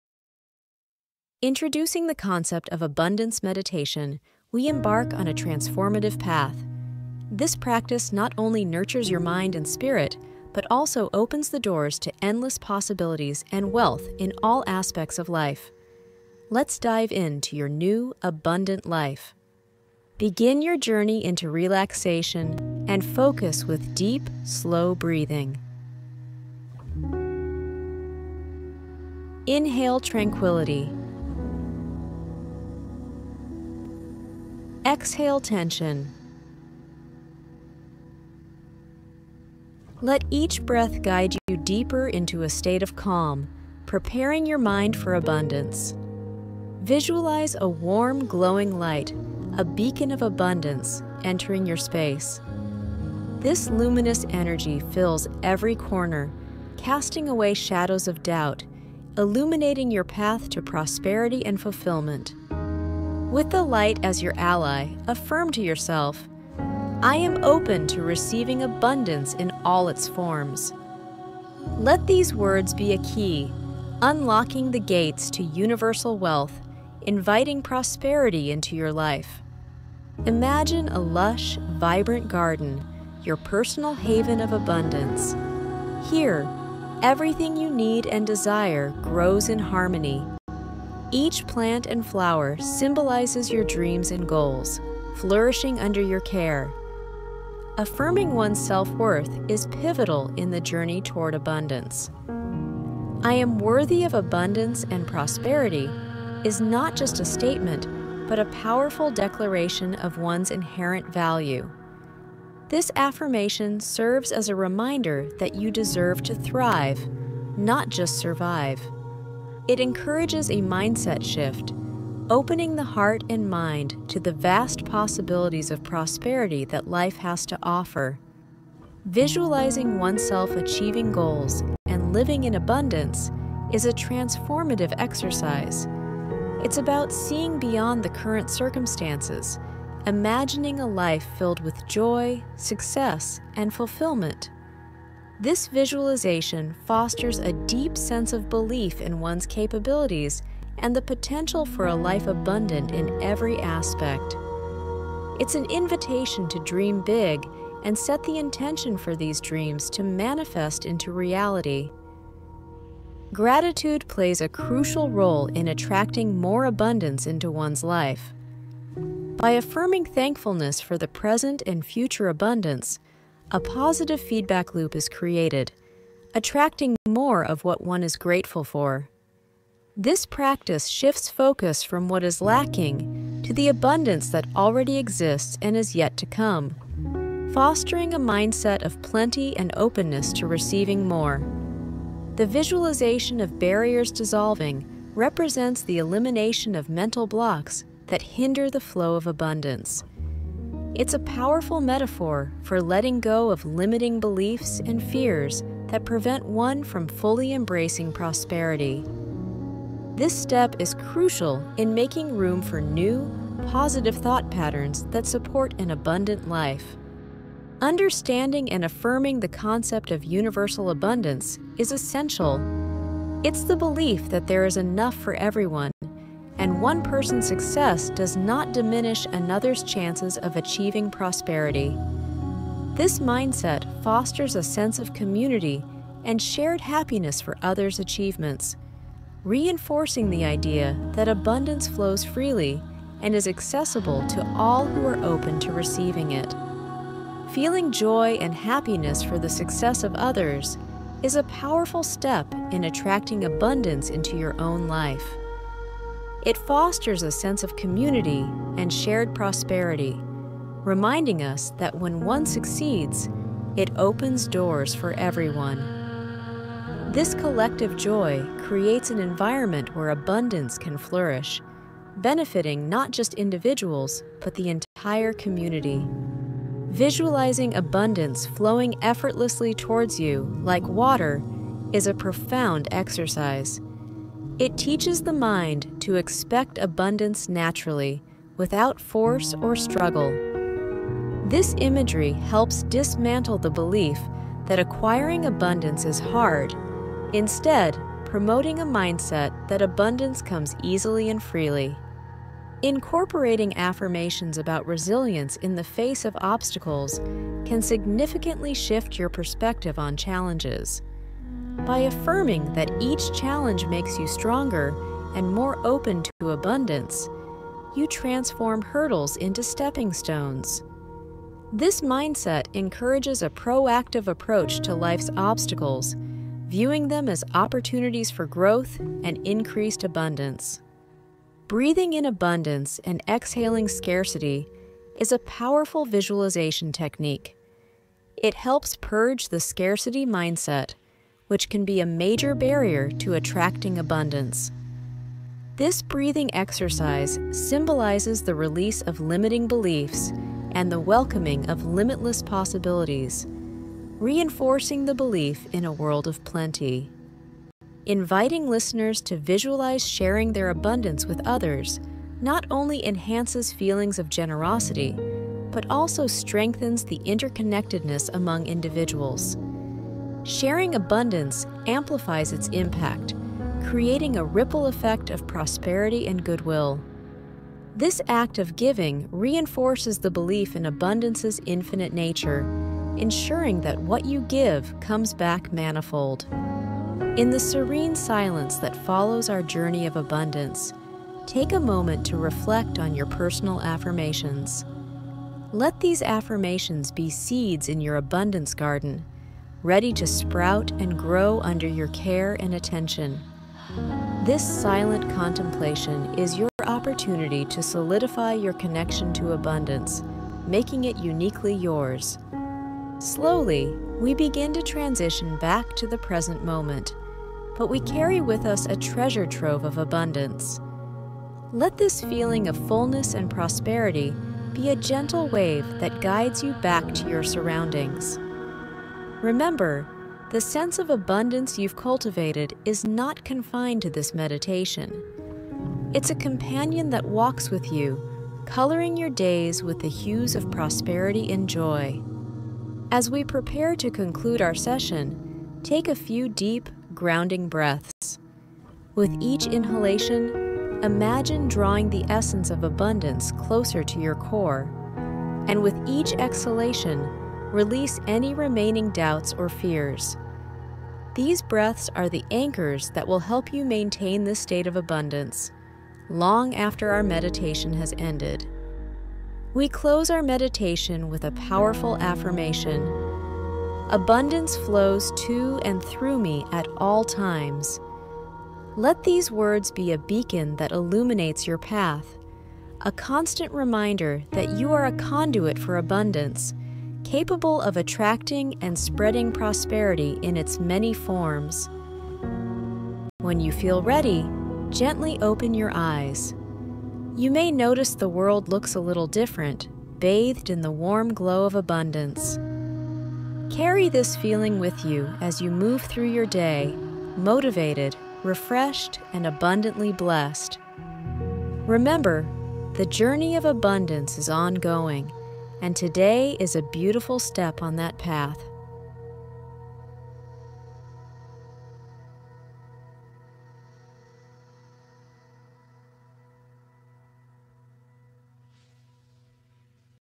Unlock Infinite Abundance: A Meditation Journey Inspired by Dr. Joe Dispenza 💰 Money Flows Easy
👉 My favorite Joe Dispenza Meditation on Amazon

Dive deep into the realms of abundance with our meditation guide inspired by the teachings of Dr. Joe Dispenza. Unlock your potential and shift your reality towards a life filled with prosperity and joy. This meditation is designed to help you harness the power of your mind to manifest abundance in all aspects of your life. Whether you're new to meditation or a seasoned practitioner, this journey will empower you to elevate your vibrational energy and attract the abundance you deserve.

🌟 Begin your transformation today and watch as the universe aligns to bring your desires to life.